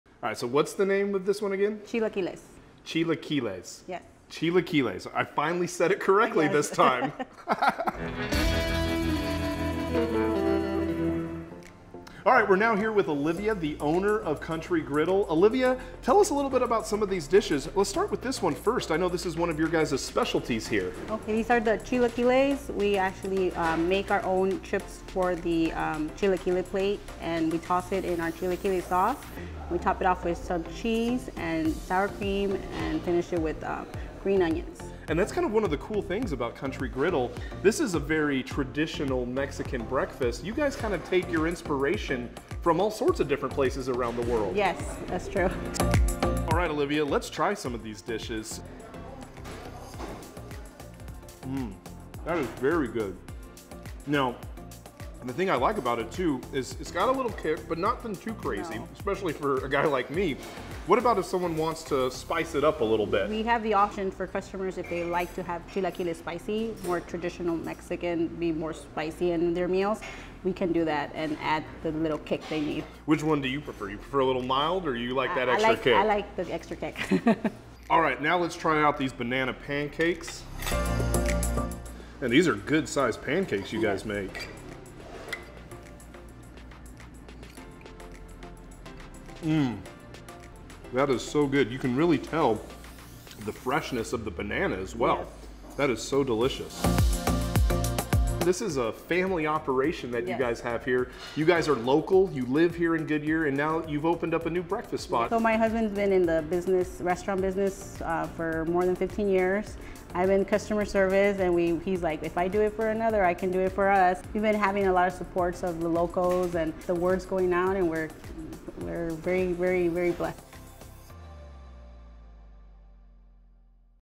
NewBusinessAlert Country Griddle is now open!
Take a look at Goodyear's newest breakfast and lunch spot, County Griddle!